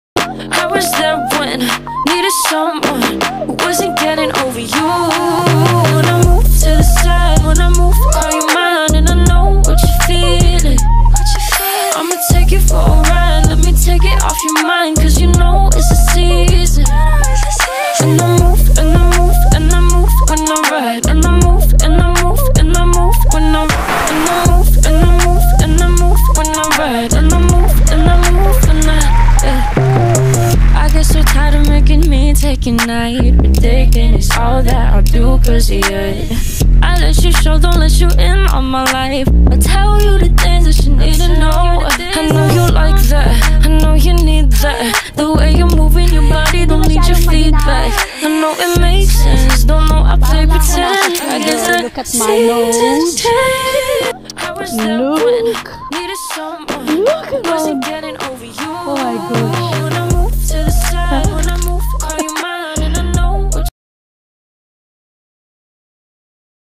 Winter Sports Skiing in Hinterströder Upperaustria/OLIVIA HOLZINGER #23
SkiiRace in Hinterströder in Upperaustria great place to explore every winter as well as in summer time for hiking&walking with friends and family